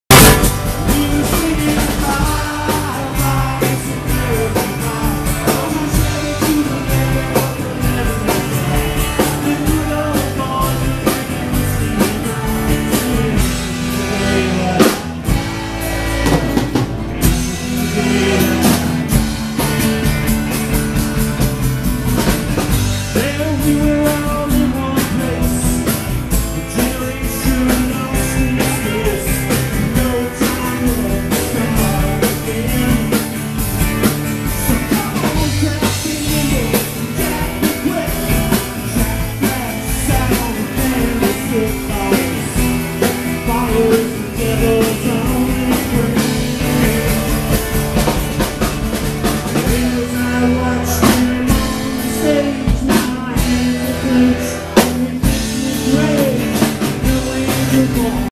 Pulse 5 - Stitch NYC March 2013
Possibly the last piece of live footage of us, it's a bit short, but that's all there is (a fitting ending/the day the music died. )